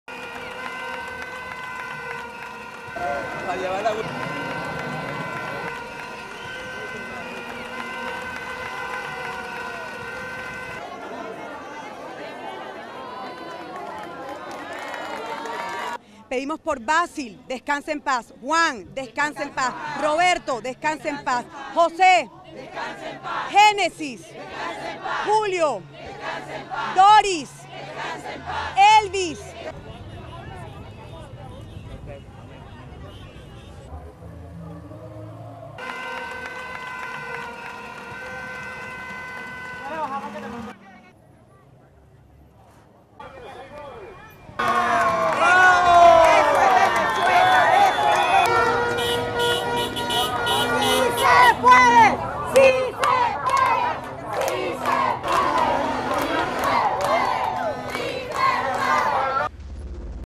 Sin comentarios: Oposición realizó concentración en honor a los caídos
Periodista: Tomás Rodríguez Acosta
Camarógrafo: Jesús Ruiz
Notiminuto.com